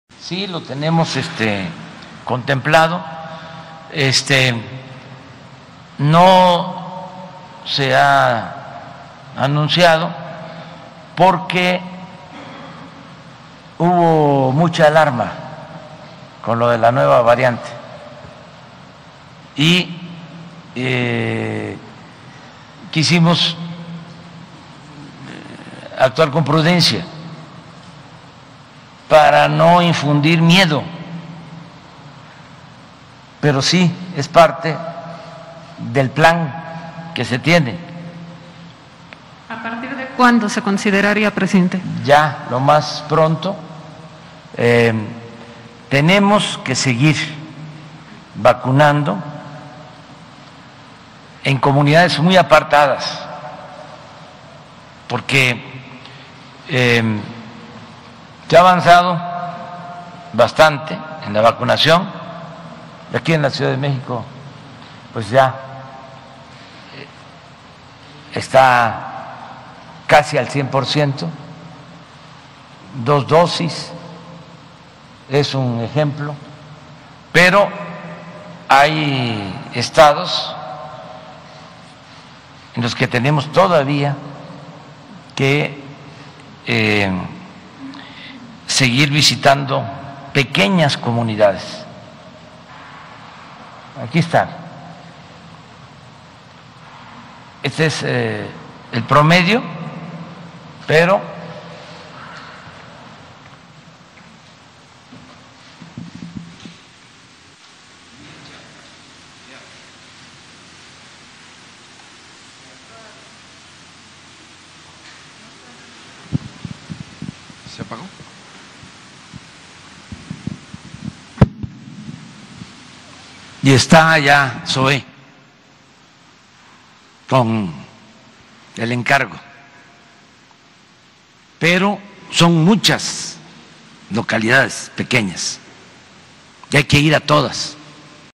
Se tiene contemplada aplicar una dosis de refuerzo contra Covid19, la nueva variante
El jefe del Ejecutivo federal confirma que se tiene contemplada aplicar una dosis de refuerzo contra Covid19, ante la alarma de la nueva variante